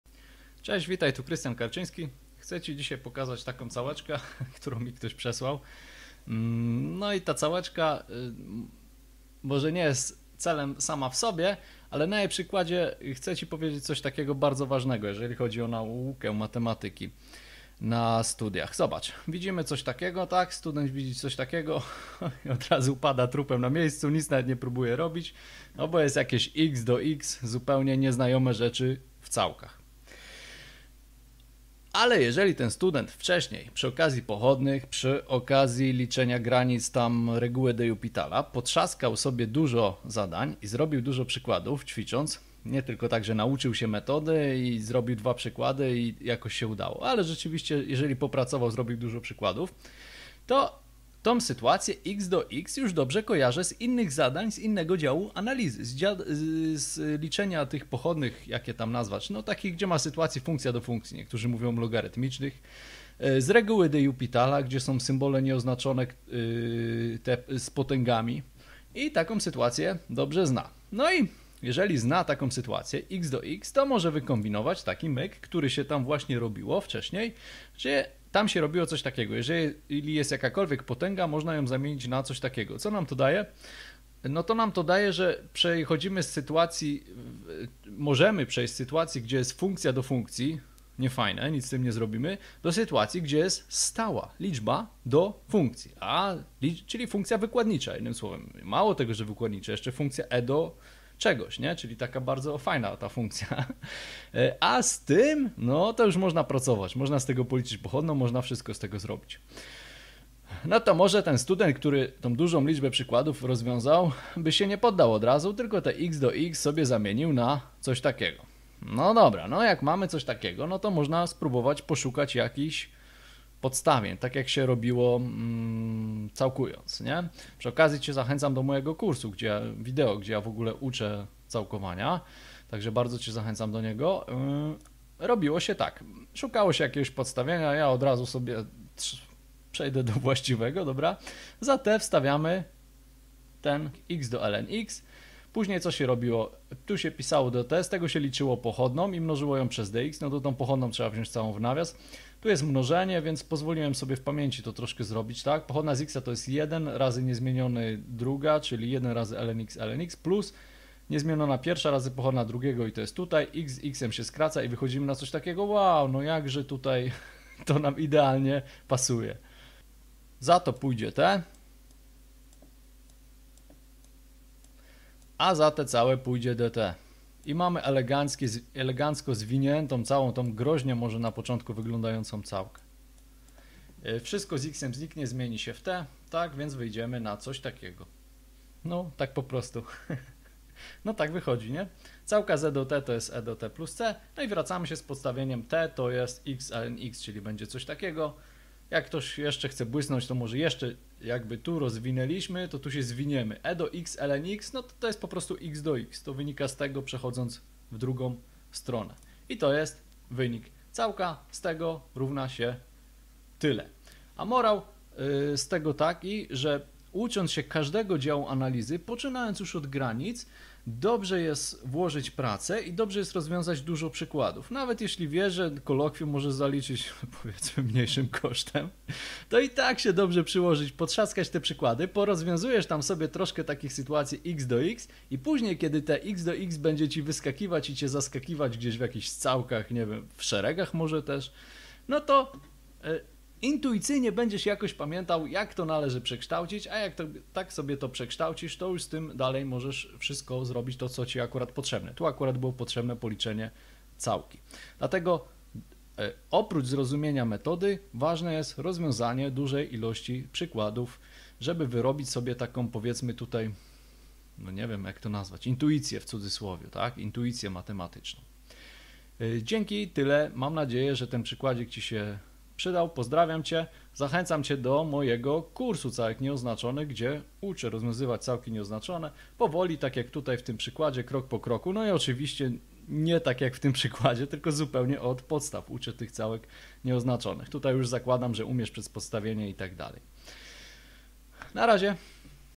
Całka nieoznaczona zawierająca x^x
Czego nas może nauczyć całka nieoznaczona zawierająca x^x?

Solidna robota i zrobienie wielu przykładów w początkowych działach analizy opłaca się także w późniejszych, co widać na całce w filmiku...

Warto zapamiętać przekształcenie funkcji x^x.